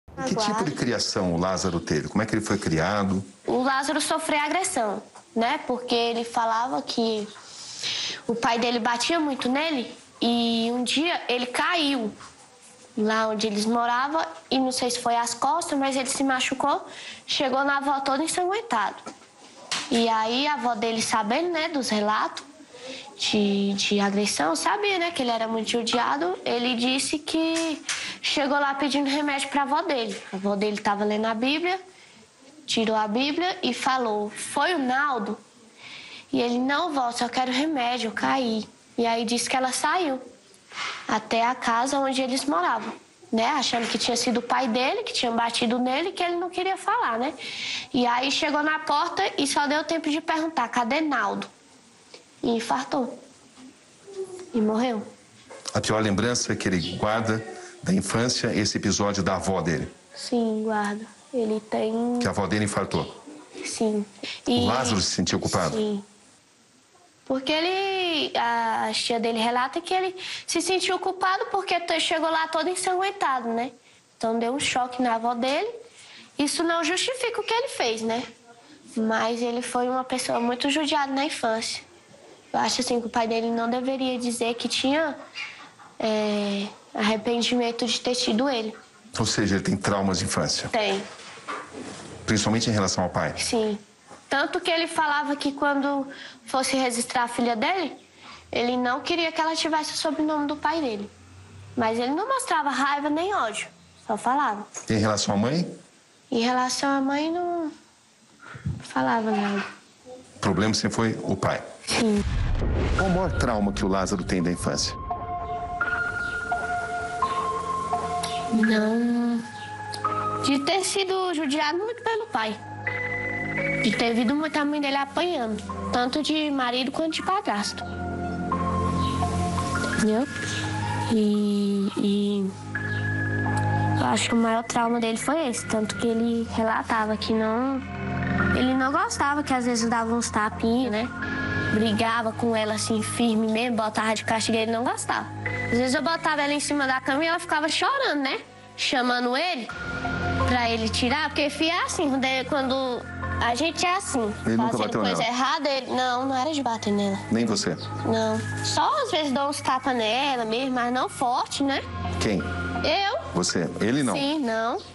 EXCLUSIVO: MULHER CONTA TRAUMAS DE INFÂNCIA DE LÁZARO
A MULHER DE LÁZARO CONTOU AO REPÓRTER ROBERTO CABRINI DETALHES SOBRE A INFÂNCIA CONTURBADA DE LÁZARO.